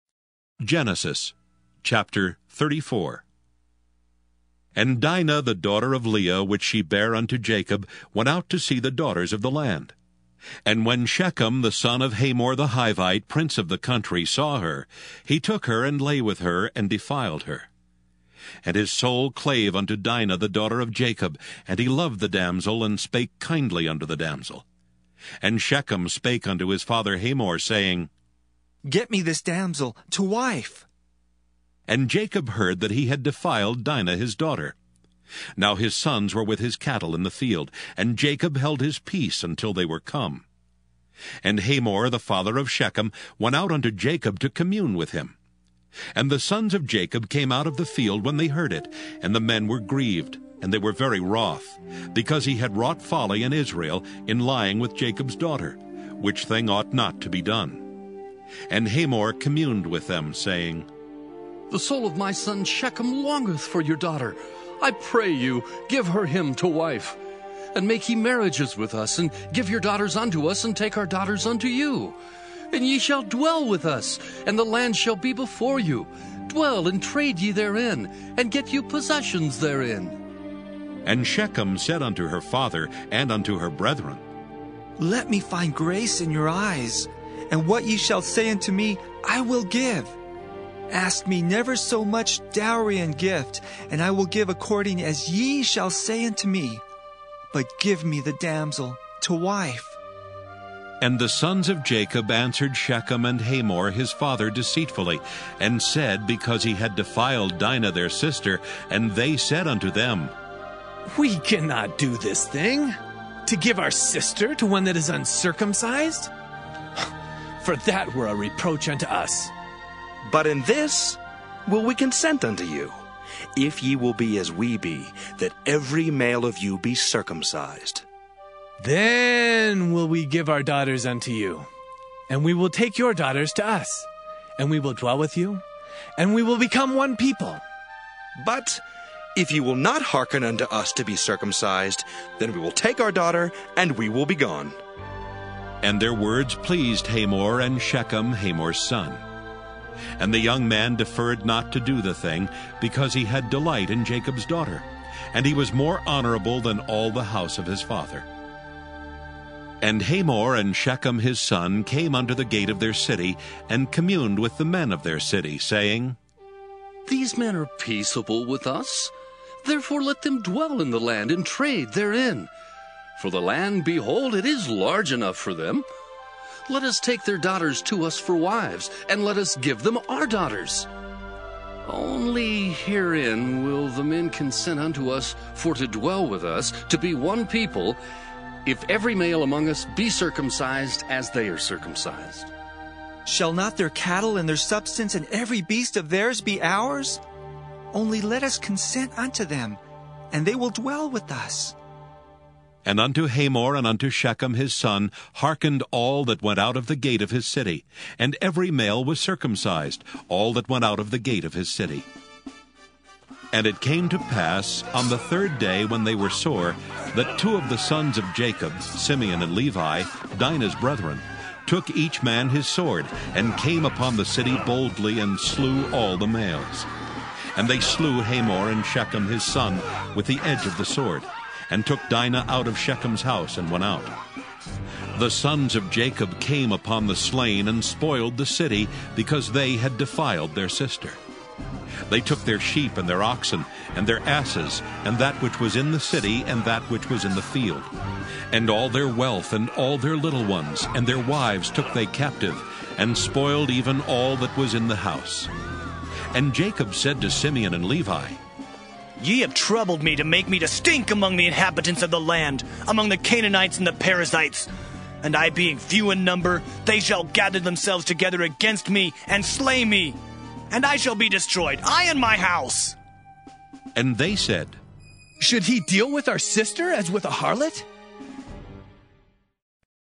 Book of Genesis - Chapter 34 - KJV Audio Bible
The Book of Genesis - Chapter 34 (Dramatized King James Version)